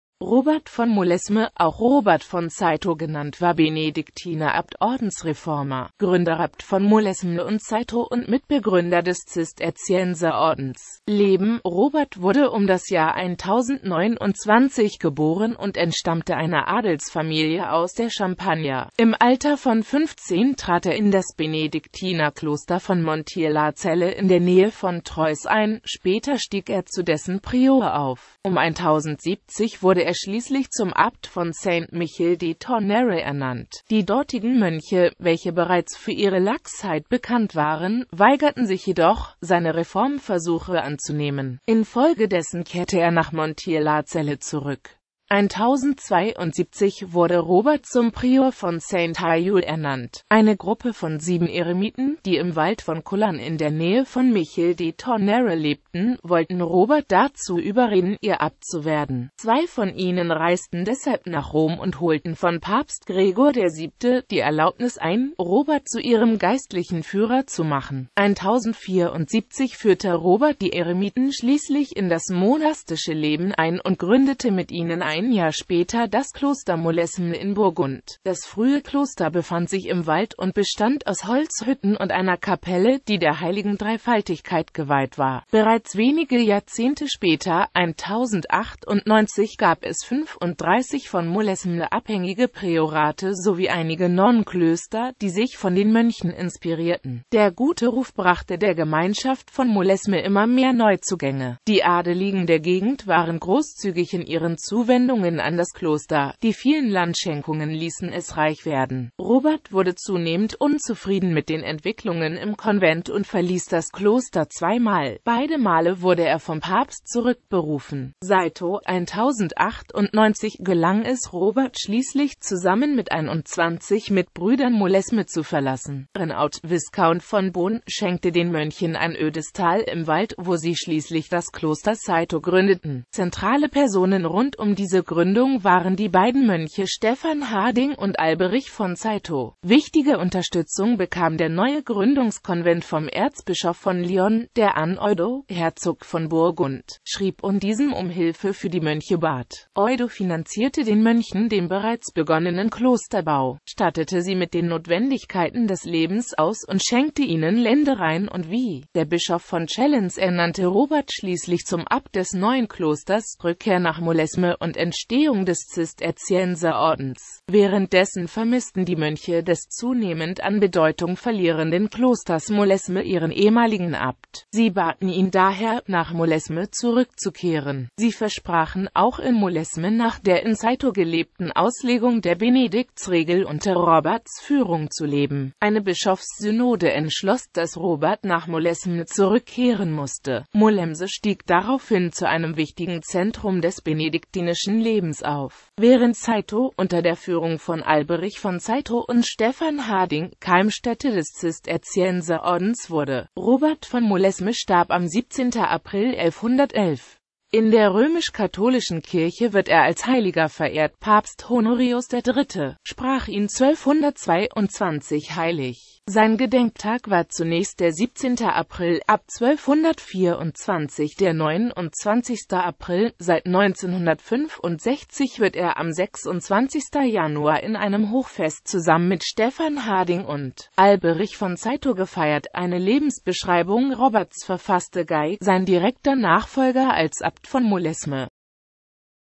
Robert von Molesme
Vielen Dank für Ihre Unterstützung:

Robert von Molesme, auch Robert von Cîteaux genannt war Benediktiner-Abt, Ordensreformer, Gründerabt von Molesme und Cîteaux und Mitbegründer des Zisterzienserordens .

Bild-Copyright-Informationen
Urheber Info: Daniel Brockpähler, reworked by TUBS